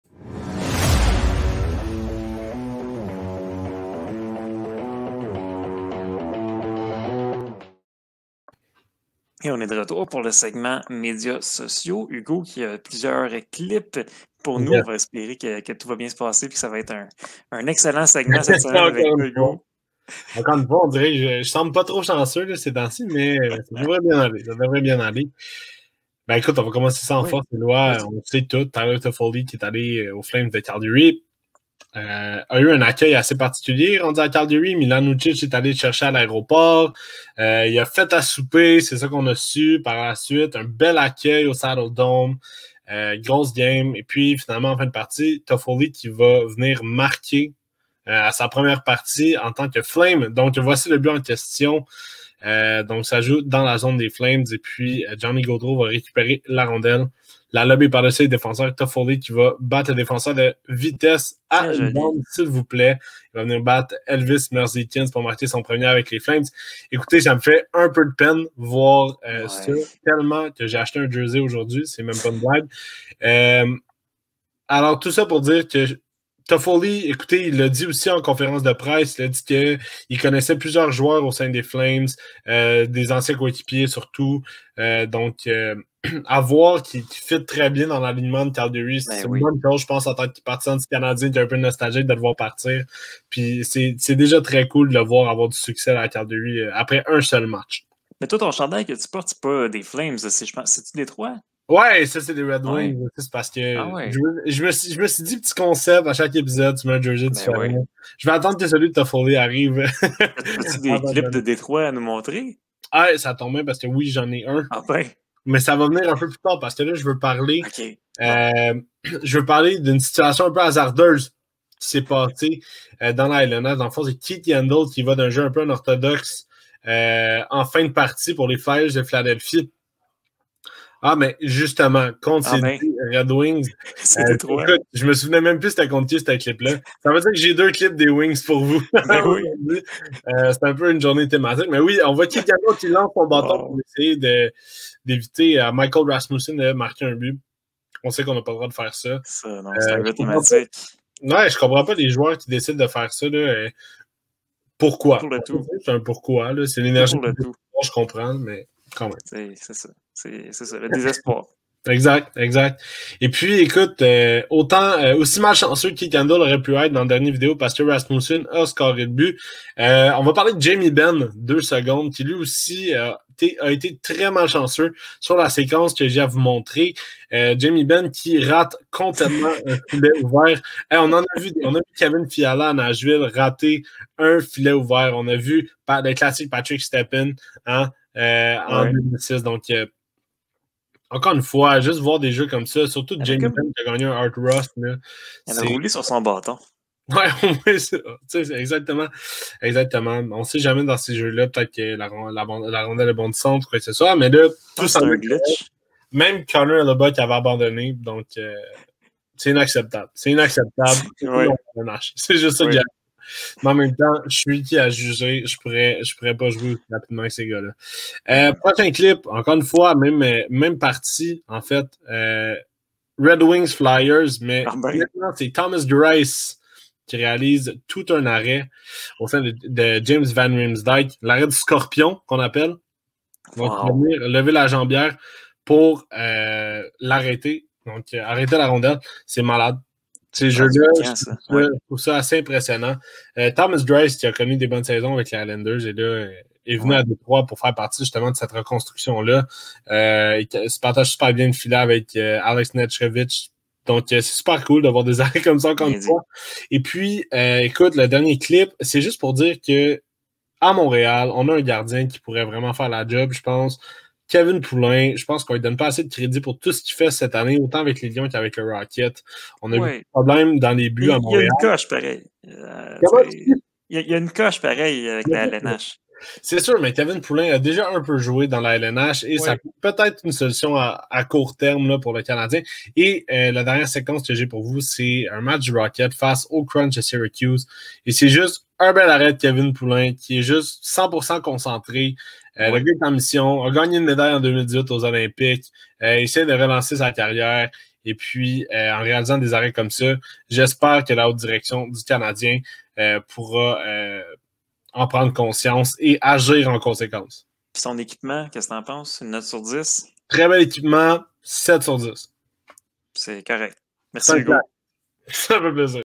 Médias sociaux - 16 février 2022
Hugo revient sur ses meilleures trouvailles sur les médias sociaux de la dernière semaine!